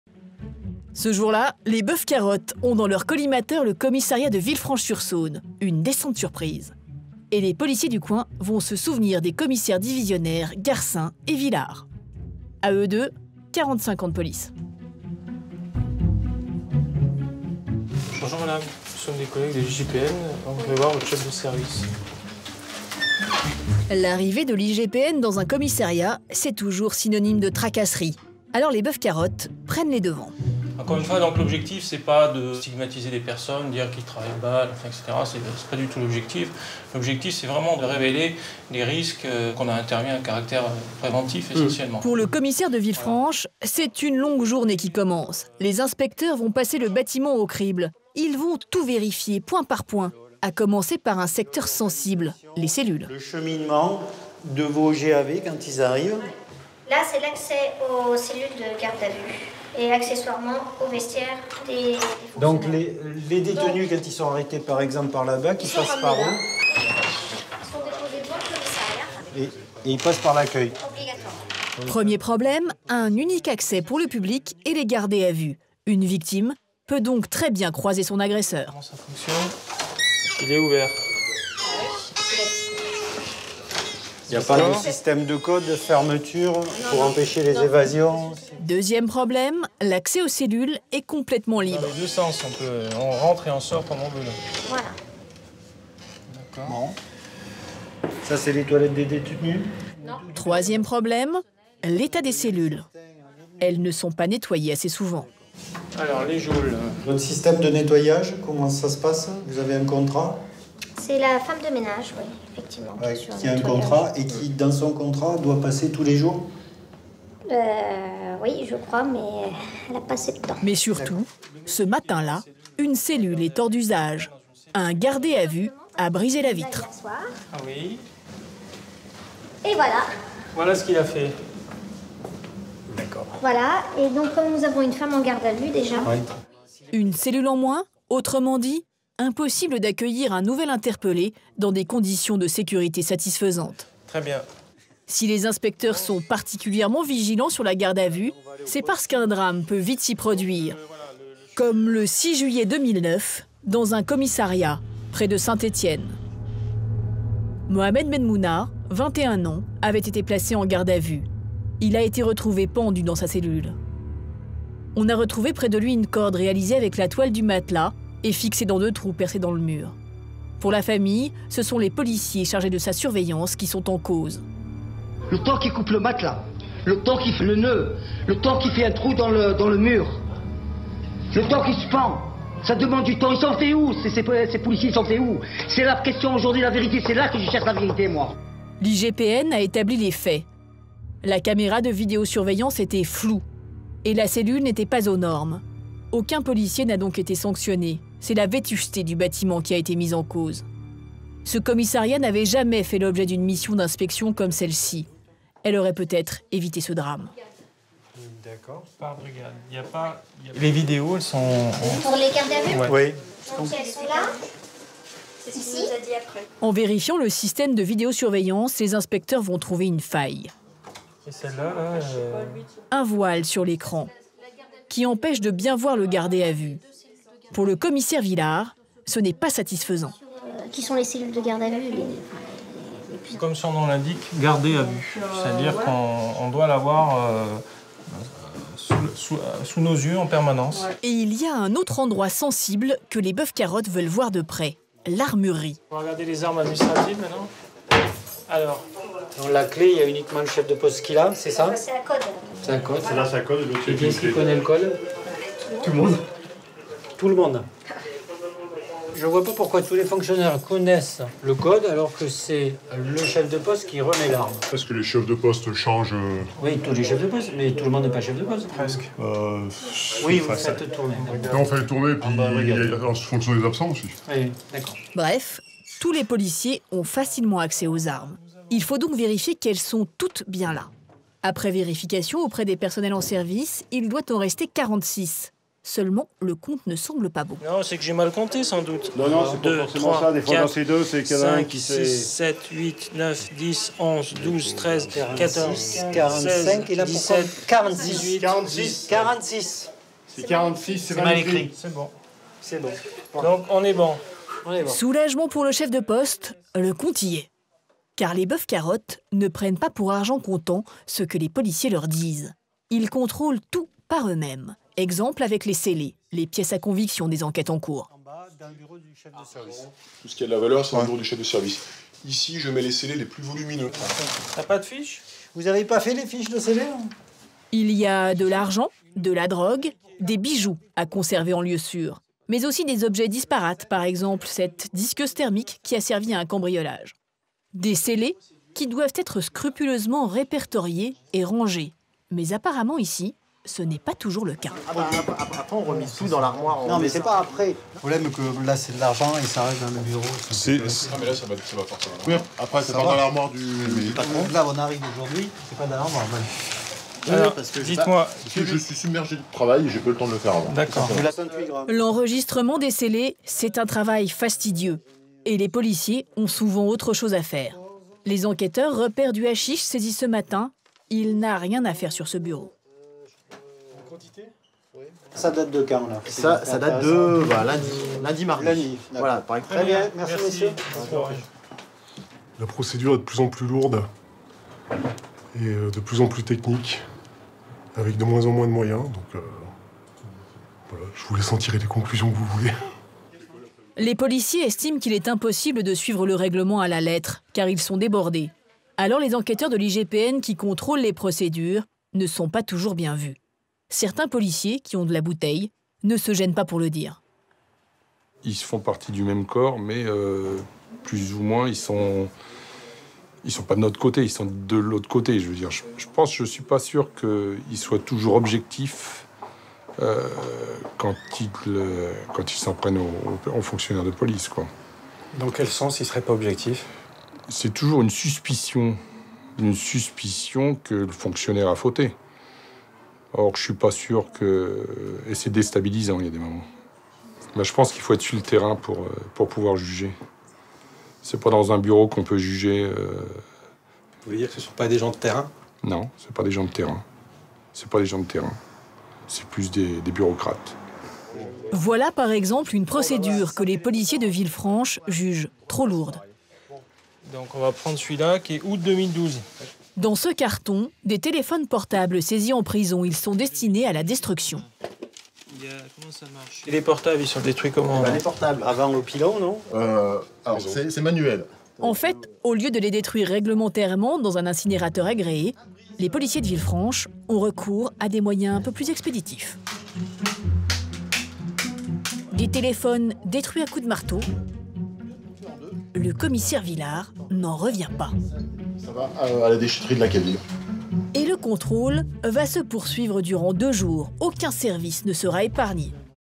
Descente surprise de l'IGPN dans un commissariat, ils inspectent tout!
Voir débarquer l'IGPN, c'est un peu comme recevoir la visite de l'inspecteur du fisc… ça ne sent pas bon ! Ces policiers se souviendront de la visite de ces commissaires, qui viennent tout passer au crible.

Réalisateur : Christophe Busché